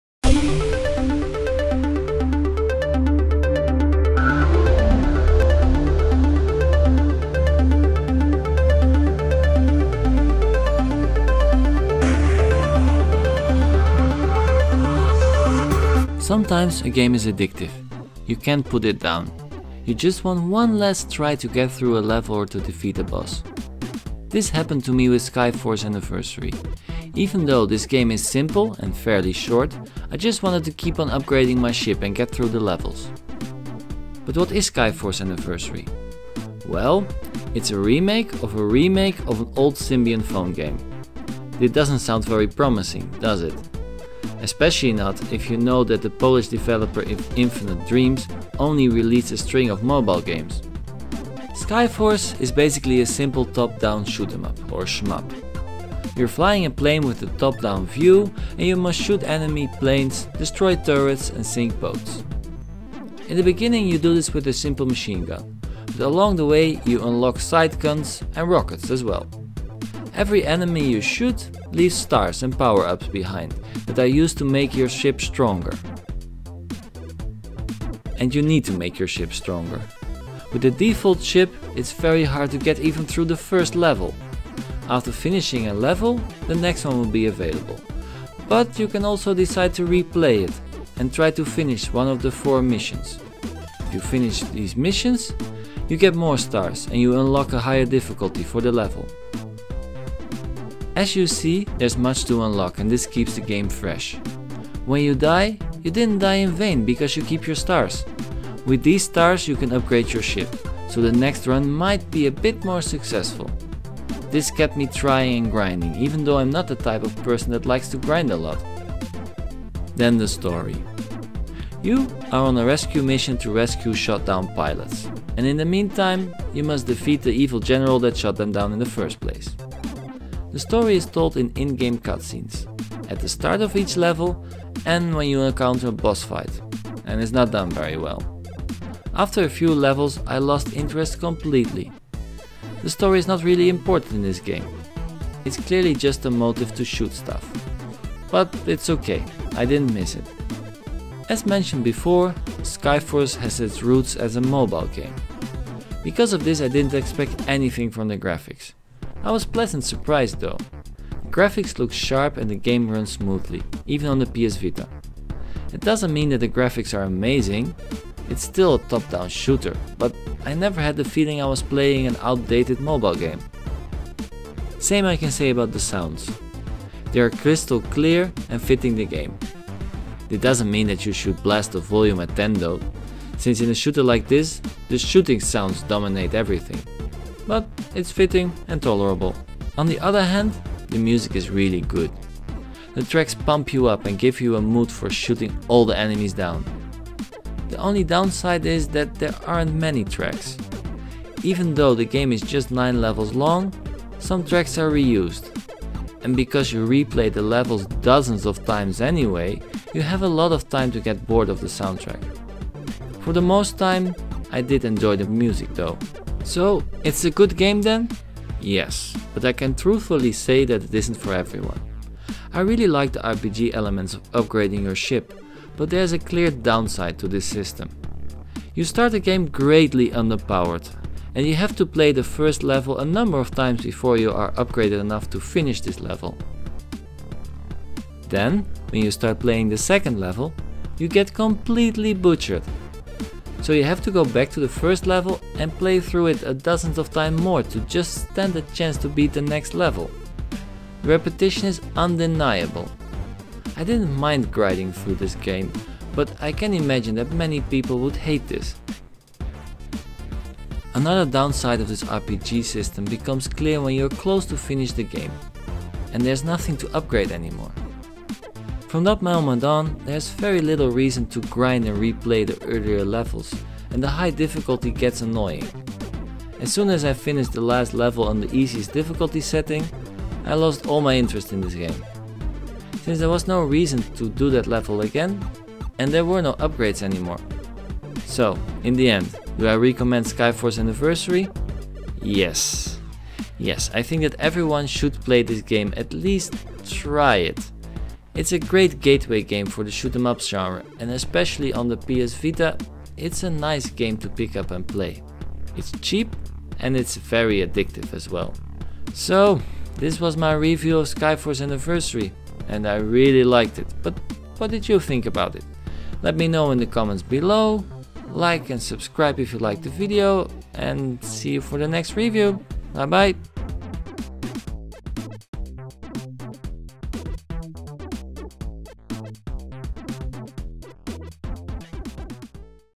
Review 14: Sky Force Anniversary (PS vita/PS4)
My review about Sky Force Anniversary on the Playstation 4, the playstation Vita and a whole bunch of other systems. If you want to hear my opinion, watch the review and comment if you like!

Soundtrack by Jacek Dojwa.